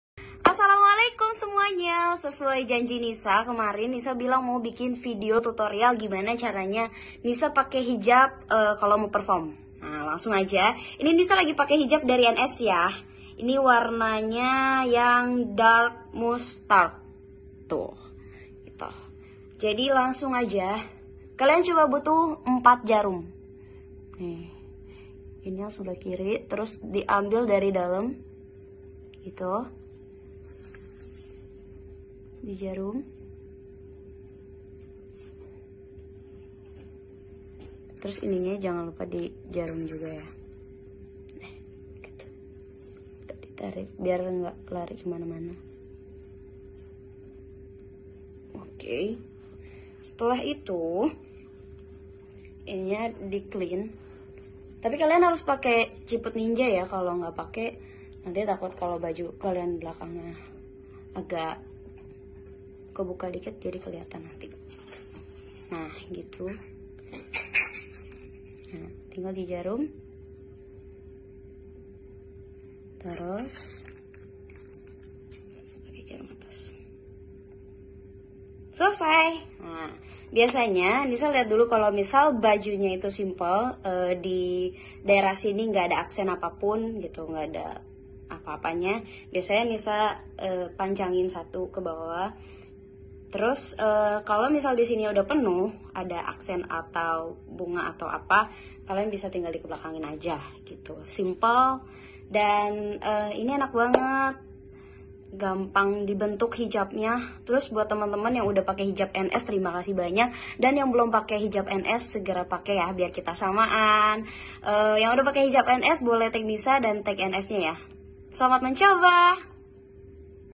Tutorial hija simpel praktis dan mudah
Tutorial hijab mudah dan simpel, cocok buat di mn aja.
Smoga bermanfaat